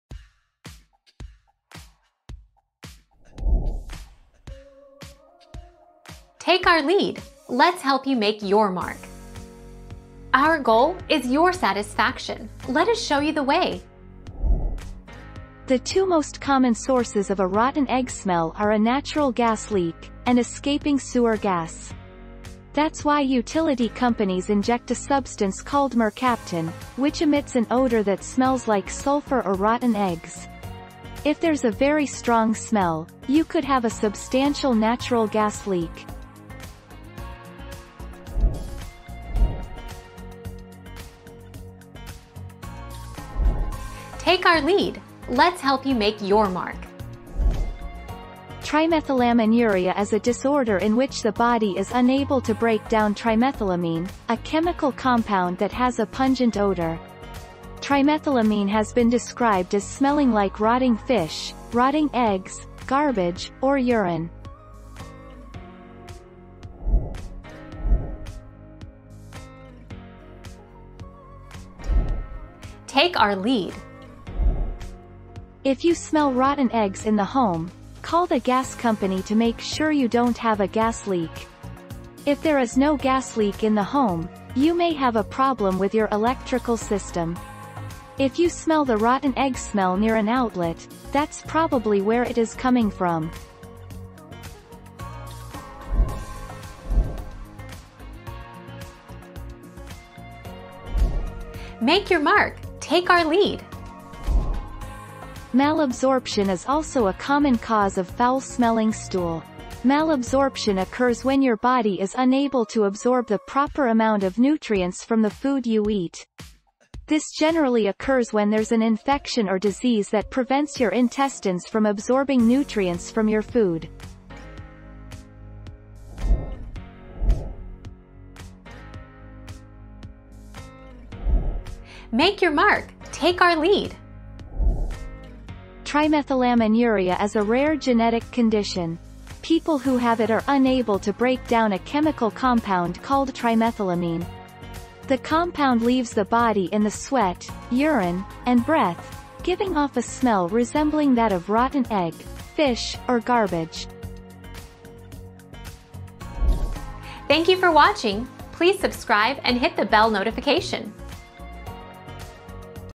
What does it mean when you smell sulfur?
00:00 - What does it mean when you smell sulfur?
00:40 - What disease makes you smell like rotten eggs?
01:08 - What do you do if you smell rotten eggs?
01:37 - Why do bowel movements smell like sulfur?
02:07 - What does it mean when your sweat smells like sulfur?

Laura S. Harris (2021, January 8.) What does it mean when you smell sulfur?
    AskAbout.video/articles/What-does-it-mean-when-you-smell-sulfur-203835